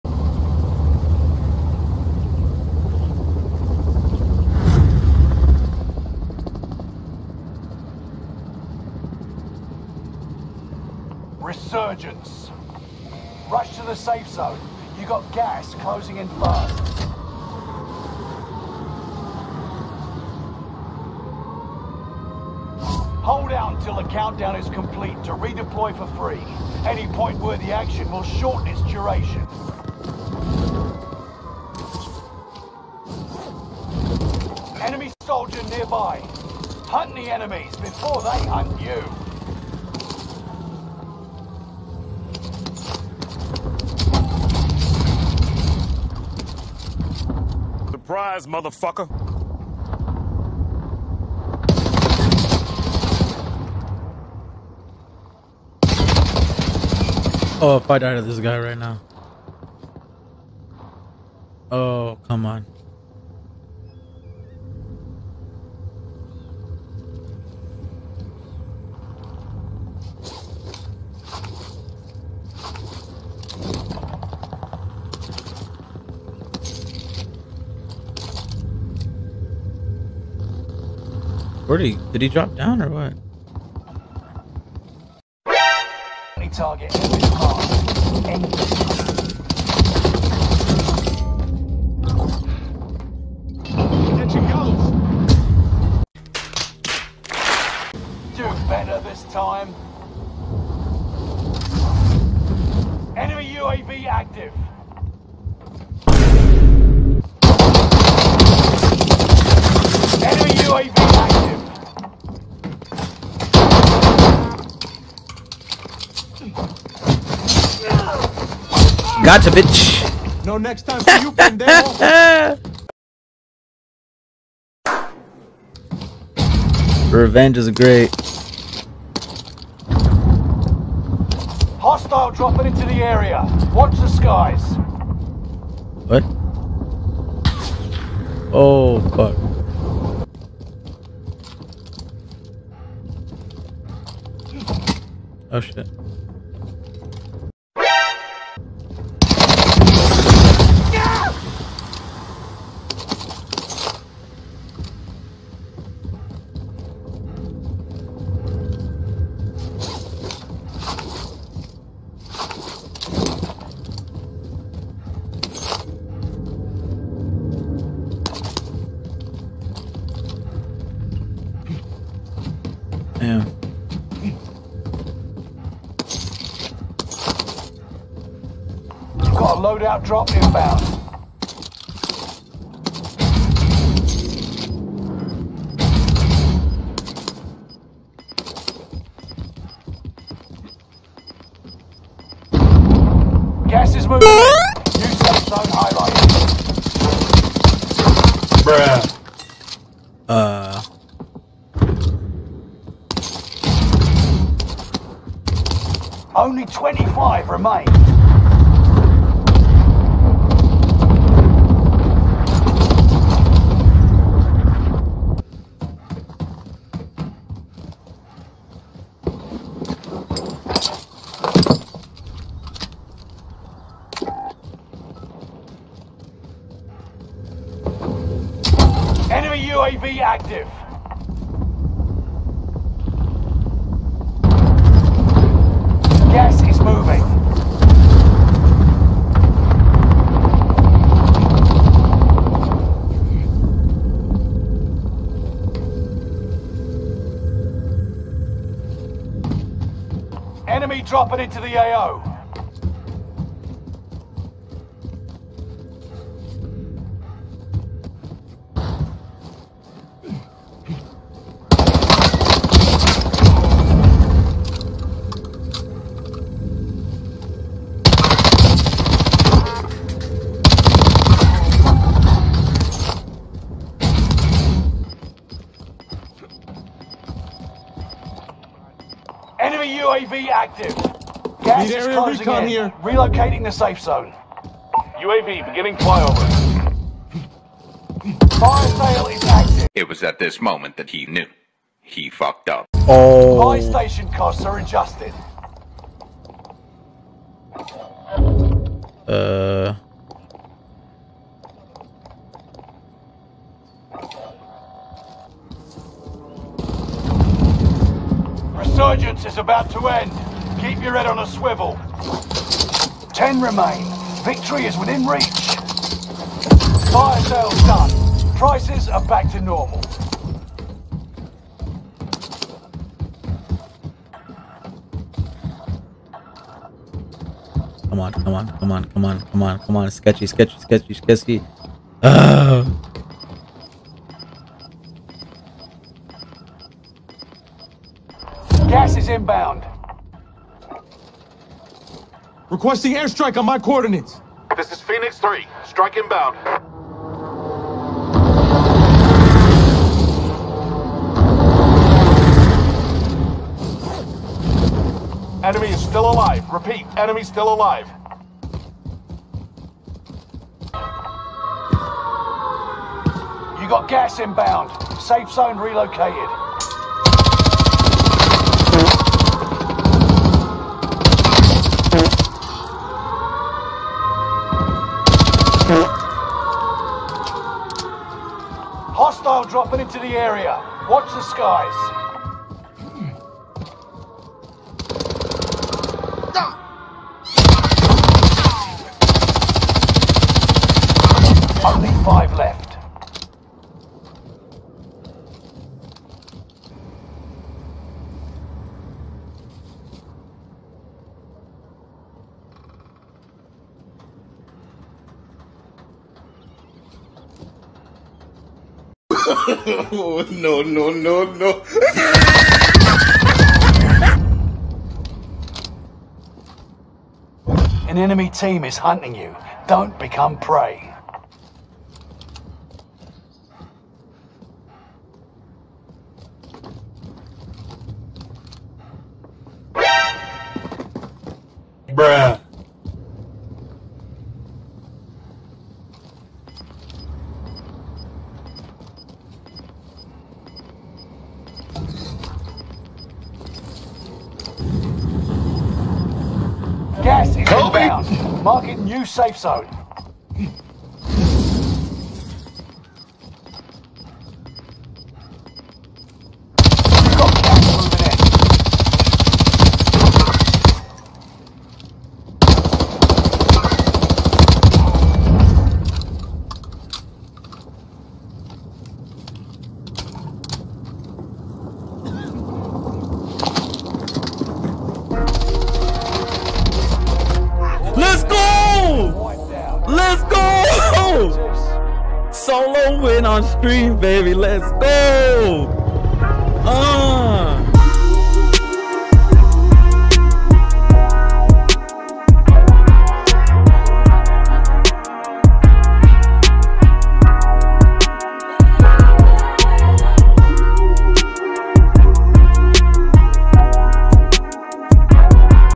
My First ever Solo's win on Stream EVER!!!
Watch me get my first ever SOLOS win on my recent livestream.
Huge shoutout to wicked cushions for making super comfortable earcups
Microphone I use: Fifine k690 USB Microphone